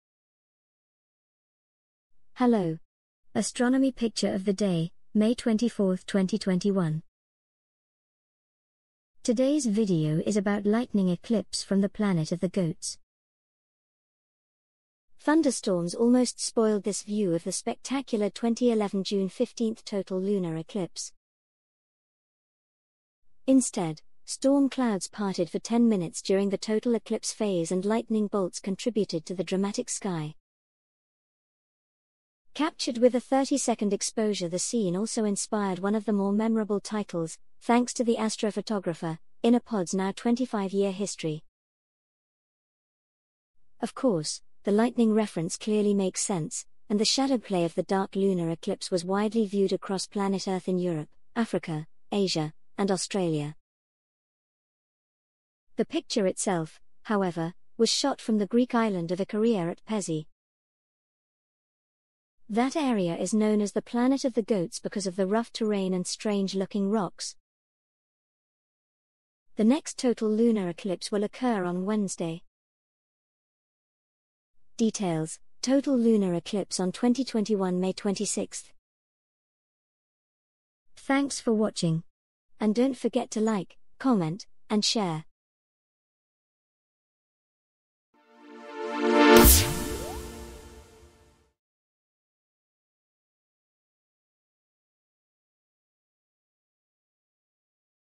APOD: 2021-05-24 - Lightning Eclipse from the Planet of the Goats (Narrated by Emma)
Astronomy Picture of the Day - May 24th, 2021 - Lightning Eclipse from the Planet of the Goats (Narrated by Emma)

Thunderstorms almost spoiled this view of the spectacular 2011 June 15 total lunar eclipse. Instead, storm clouds parted for 10 minutes during the total eclipse phase and lightning bolts contributed to the dramatic sky. Captured with a 30-second exposure the scene also inspired one of the more memorable titles (thanks to the astrophotographer) in APOD's now 25-year history.  Of course, the lightning reference clearly makes sense, and the shadow play of the dark lunar eclipse was widely viewed across planet Earth in Europe, Africa, Asia, and Australia. The picture itself, however, was shot from the Greek island of Ikaria at Pezi. That area is known as "the planet of the goats" because of the rough terrain and strange looking rocks.  The next total lunar eclipse will occur on Wednesday.   Details: Total Lunar Eclipse on 2021 May 26